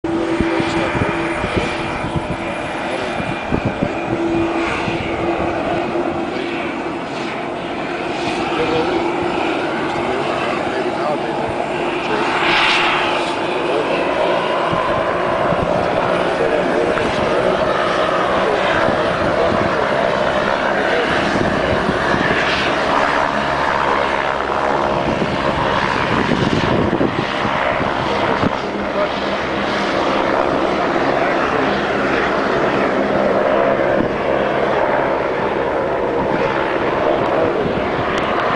Yorba Linda Fire 2 of 2
This is the fire in Yorba Linda CA on 11/15/08 taken from the back of the Ayers Suites Hotel where I was staying